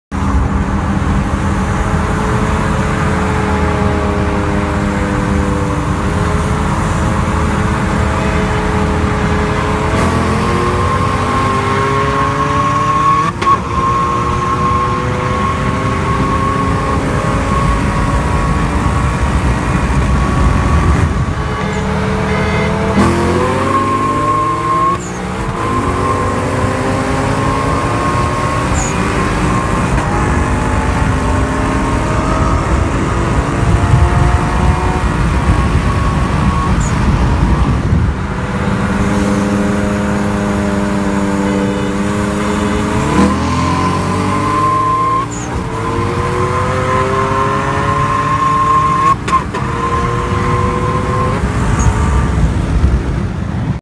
2015 mustang gt vs 08 335i
Adam 15 mustang gt intake exhaust tune vs tony 335i boltons tune